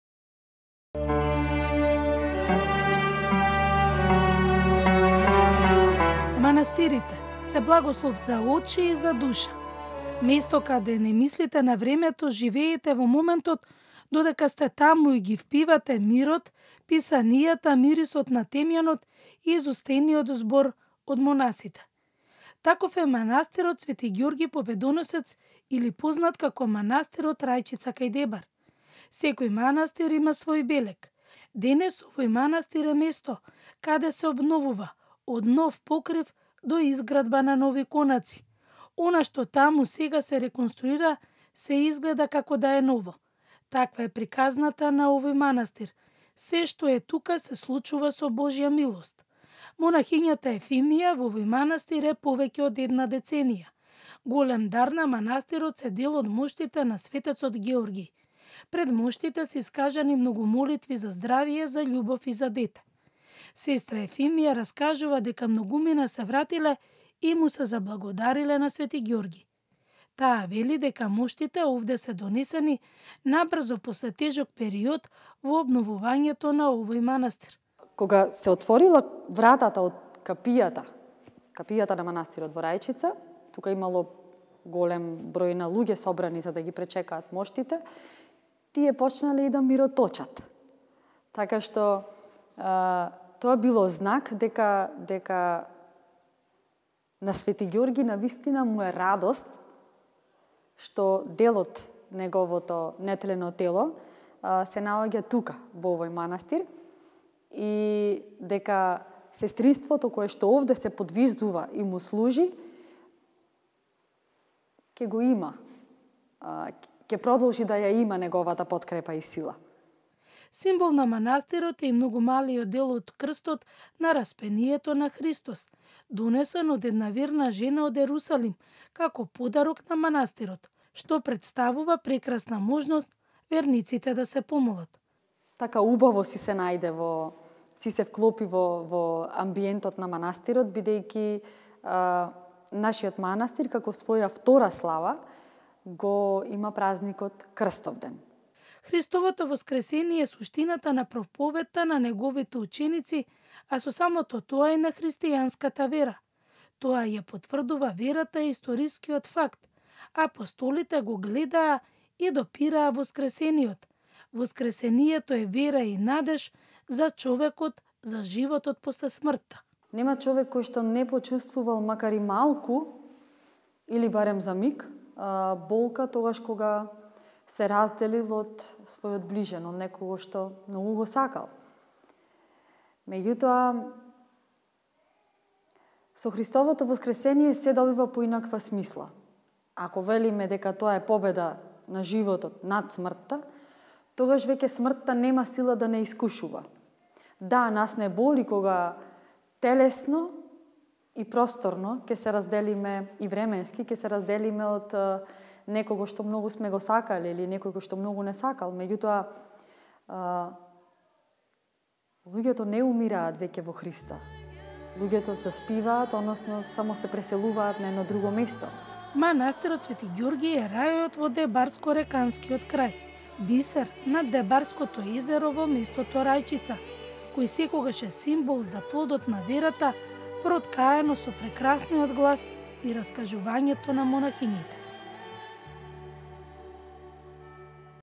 Манастирот Свети Георгиј посетен од бројни верници и за велигденските празници
Манастирите се благослов за очи и за душа, место каде не мислите на времето, живеете во моментот додека сте таму и ги впивате мирот, писанија, мирисот на темјанот и изустениот збор од монасите.